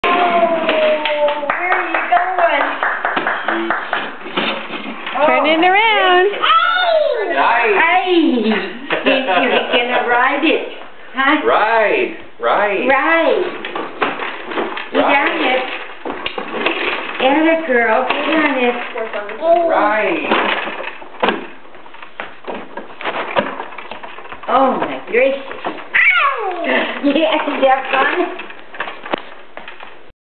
New Toy
Paige trying to figure out her new bike